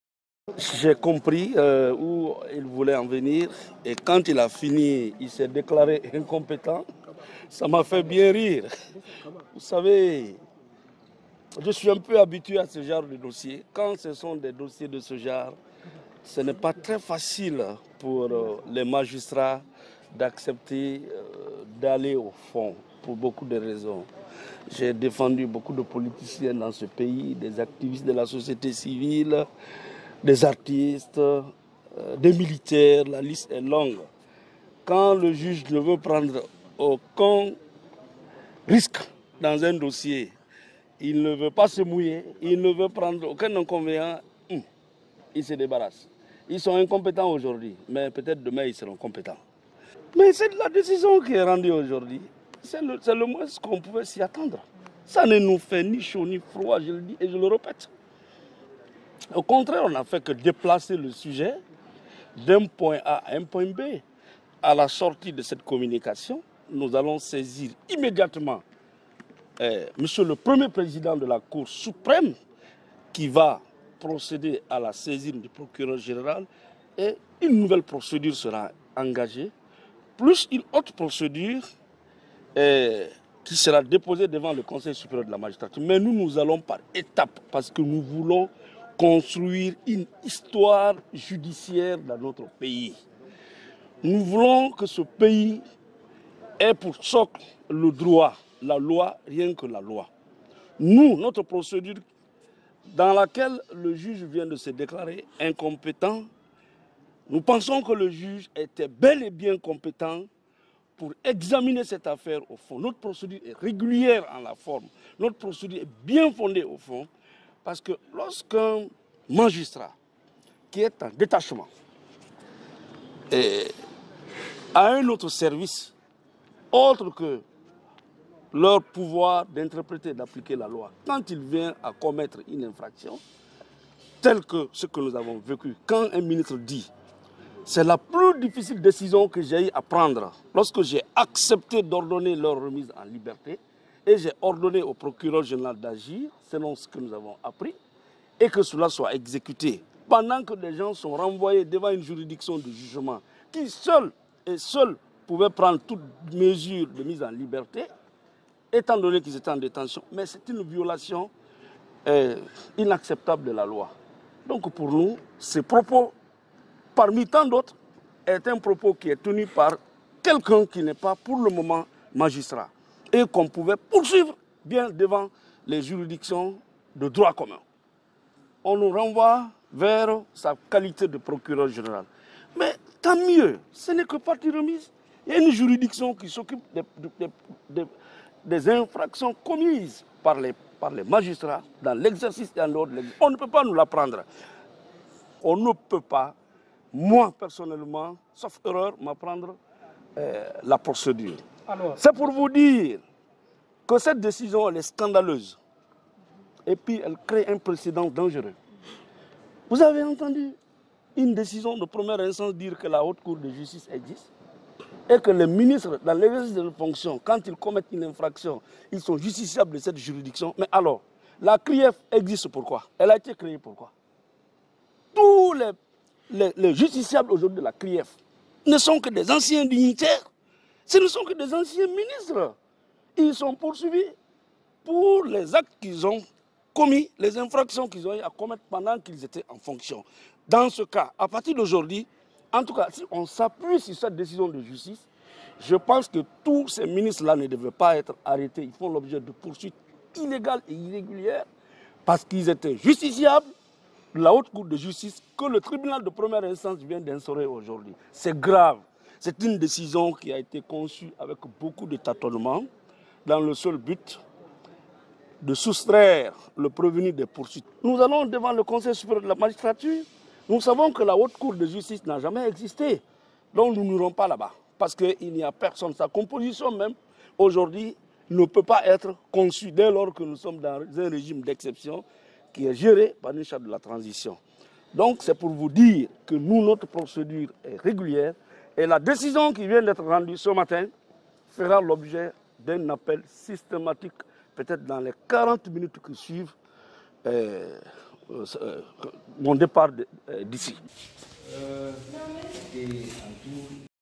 «Incompétence» du TPI de Dixinn à juger Charles Wright: « c'est un précédent dangereux », dit Me Béa
Comme annoncé précédemment, le procès en première instance du ministre de la justice et des droits de l’homme, Alphonse Charles Wright, a connu son épilogue ce lundi, 3 juillet 2023, devant le tribunal correctionnel de Dixinn (délocalisé à la Cour d’appel de Conakry). Le Garde des Sceaux était poursuivi pour « dénonciation calomnieuse, diffamation, injures publiques, violences et voies de faits et abus d’autorités » par des leaders du Front national pour la défense de la constitution (FNDC) devant cette juridiction. Mais, finalement, le tribunal s’est déclaré « incompétent » de le juger à cause de sa double qualité de magistrat et de ministre.

Au sortir de la salle d’audience, Me Salifou Béavogui, avocat de la partie civile dans cette affaire, a dénoncé une « décision est scandaleuse » qui crée un « précédent dangereux » en Guinée.

« Dès que le président a commencé la lecture de cette décision de justice, j’ai compris où il voulait en venir. Et, quand il a fini, il s’est déclaré incompétent, ça m’a fait bien rire. Vous savez, quand c’est des dossiers de ce genre, ce n’est très facile pour les magistrats d’accepter d’aller au fond pour beaucoup de raisons… Quand le juge ne veut prendre aucun risque dans un dossier, il ne veut pas se mouiller, il s’en débarrasse. Ils sont incompétents aujourd’hui, mais peut-être demain ils seront compétents… Mais, ça ne nous fait ni chaud, ni froid. A la sortie de cette communication, nous allons saisir immédiatement monsieur le premier président de la Cour suprême qui va procéder à la saisine du procureur général et une nouvelle procédure sera engagée, plus une autre procédure qui sera déposé devant le conseil supérieur de la magistrature. Nous nous allons par étape, parce que nous voulons construire une histoire judiciaire dans notre pays… Nous continuons de penser que le juge était bel et bien compétent pour examiner cette affaire au fond. Parce que notre procédure est régulière en la forme, elle est bien fondée au fond… Cette décision est scandaleuse et elle crée un précédent dangereux. C’est une décision qui a été concise avec beaucoup de tâtonnement dans le seul but de soustraire le prévenu des poursuites. Cette décision fera l’objet d’un appel systématique », a déclaré Me Béa.